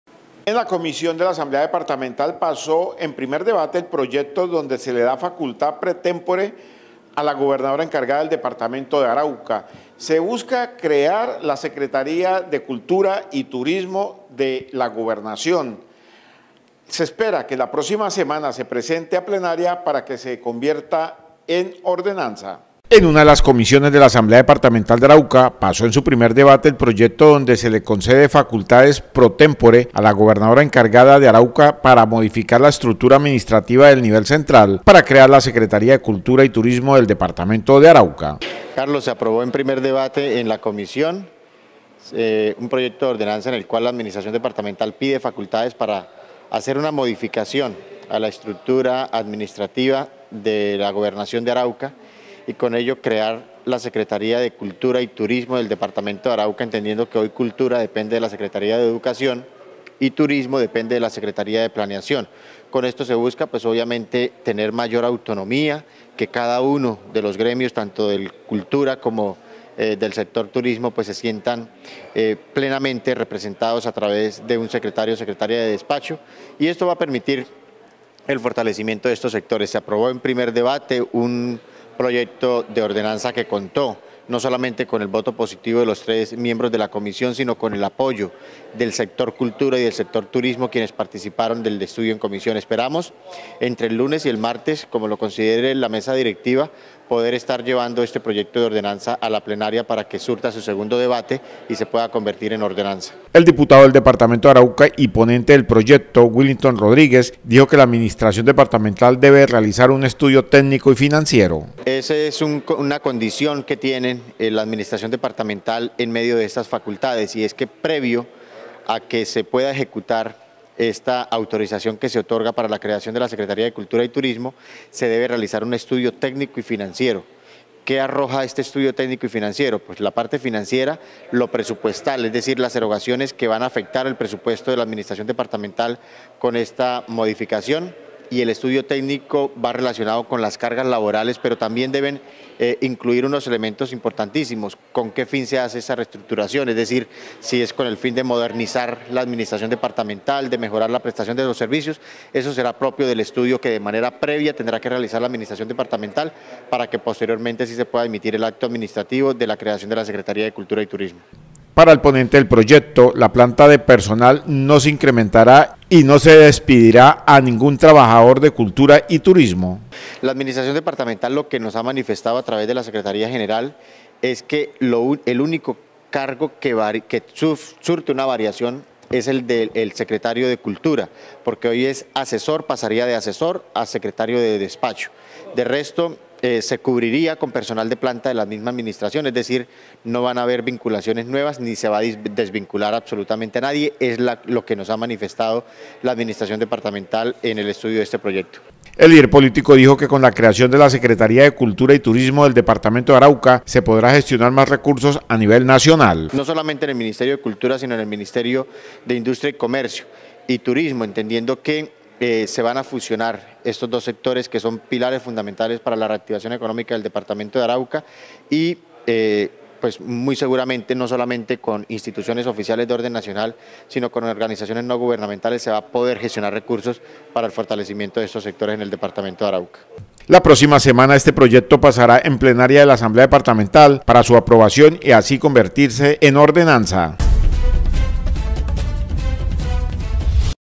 EN LA ASAMBLEA DE ARAUCA PASO EL PROYECTO DE LAS FACULTADES PRETEMPORE A LA GOBERNADORA DE ARAUCA
EN LA ASAMBLEA DEPARTAMENTAL DE ARAUCA PASO EL PROYECTO DE LAS FACULTADES PRETEMPORE A LA GOBERNADORA DE ARAUCA